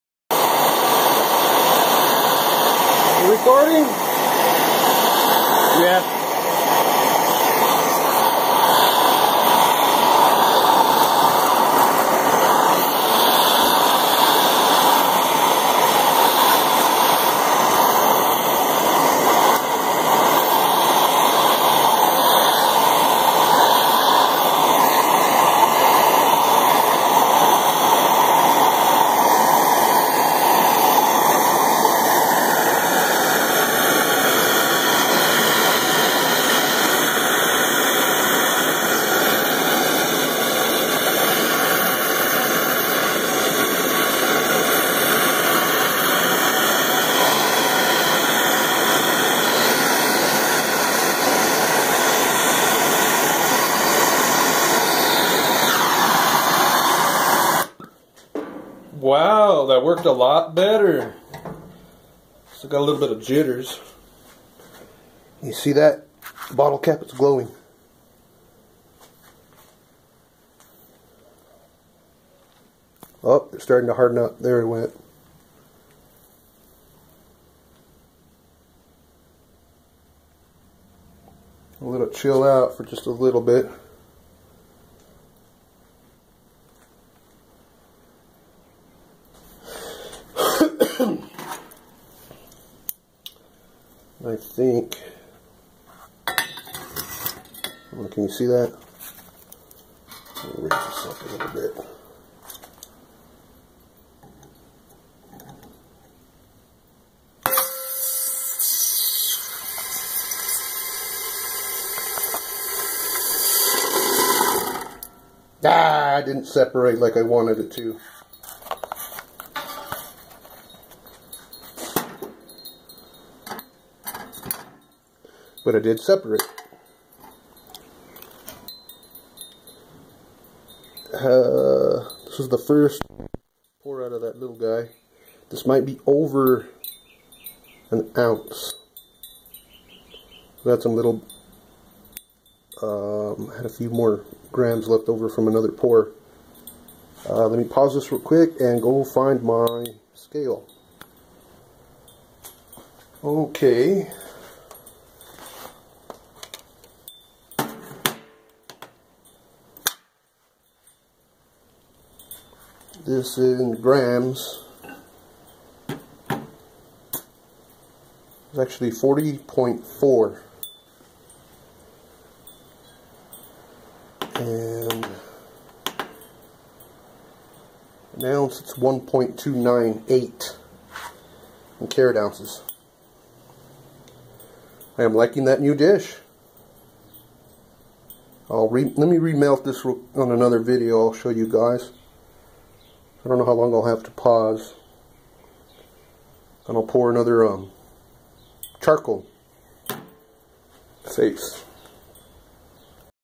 Bottle cap silver pour
First pour with the smaller crucible in the bottle cap. It is over weight by 9 grams but hay it's a keeper or melt off the 9 grams?